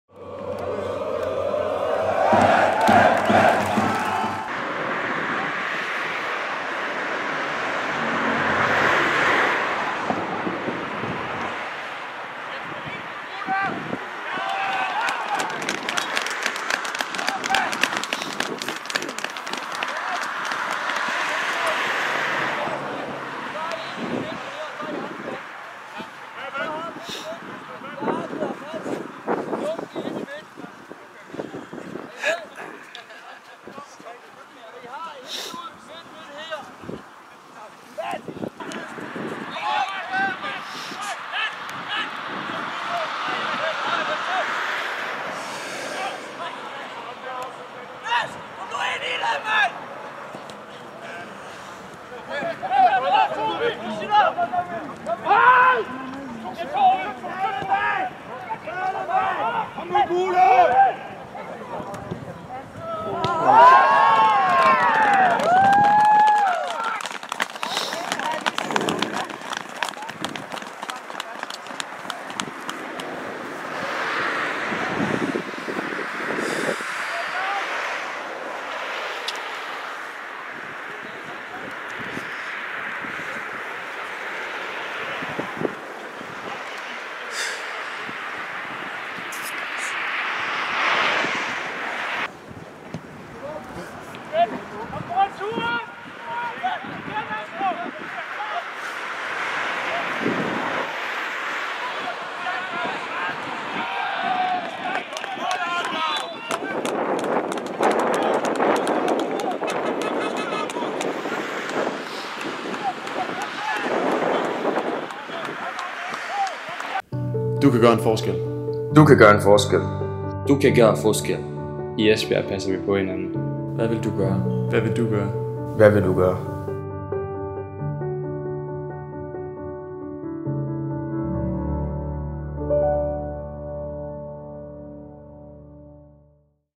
Se målene fra lørdagens test-sejr over AC Horsens
Se eller gense målene fra lørdagens 2-1 sejr over AC Horsens i forårets anden træningskamp.